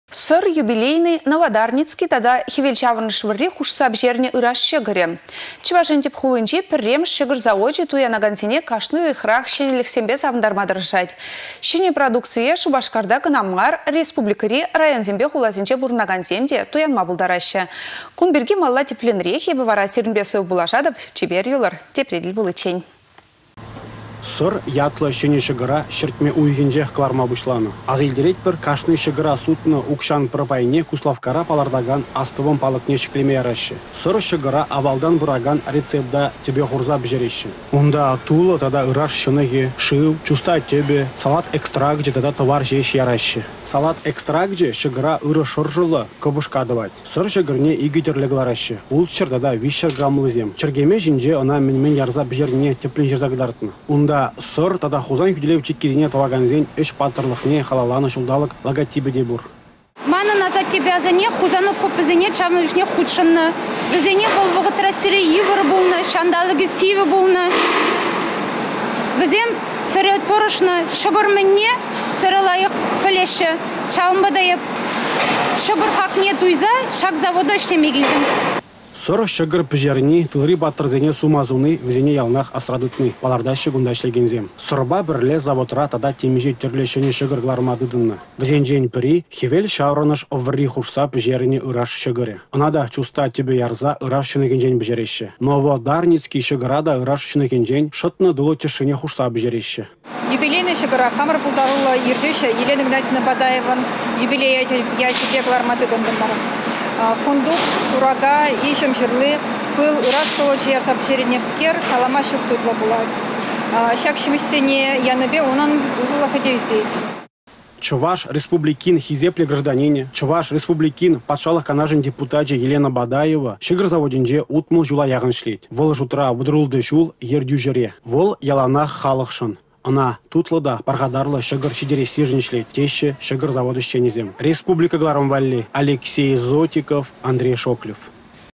Кăпăшка çăкăрпа сăйлаççĕ
«Сăр», «Юбилейный», «Новодарницкий» тата хĕвелçаврăнăш вăрри хушса пĕçернĕ ыраш çăкăрĕ. Чăваш Ен тĕп хулинчи  1-мĕш çăкăр завочĕ туянакансене кашни уйăхрах  çĕнелĕхсемпе савăнтарма тăрăшать. Çĕнĕ продукцие Шупашкарта кăна мар, республикăри районсемпе хуласенче пурăнакансем те  туянма пултараççĕ. Кун пирки малалла тĕплĕнрех. Эпĕ вара сиренпе сывпуллашатăп.